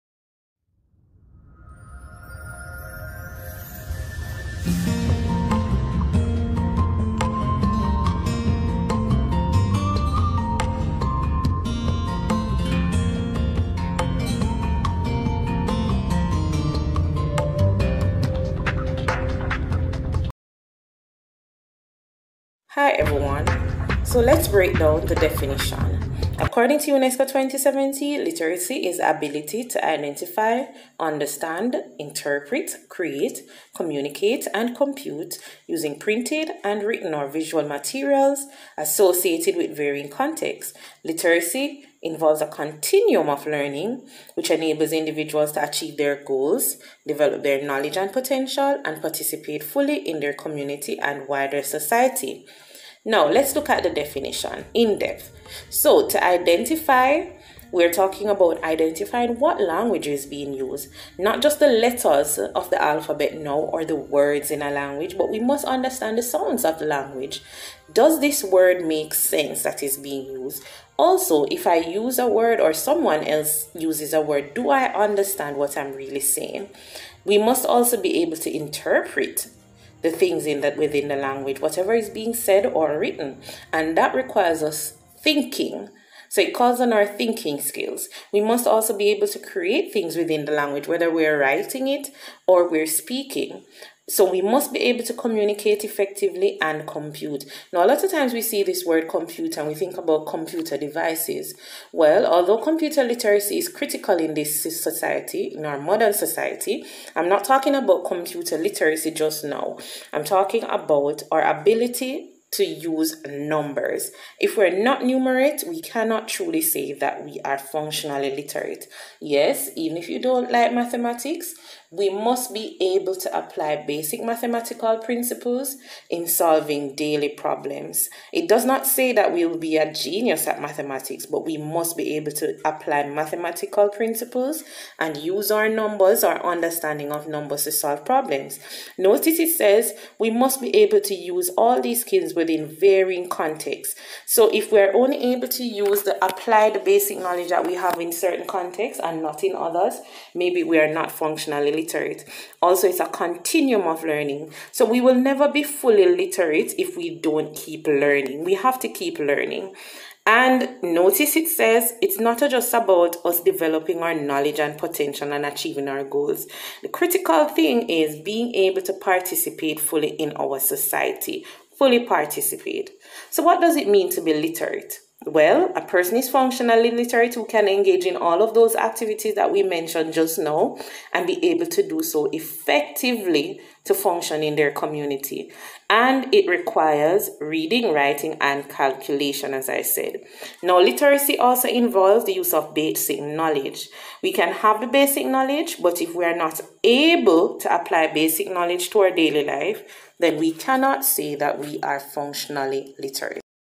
What is 'literacy'? (Part 1 of 2)
Today we go back and look at what 'literacy' is and what it means to be literate. In our next session, we will look the skills that we all have to master to be functionally literate.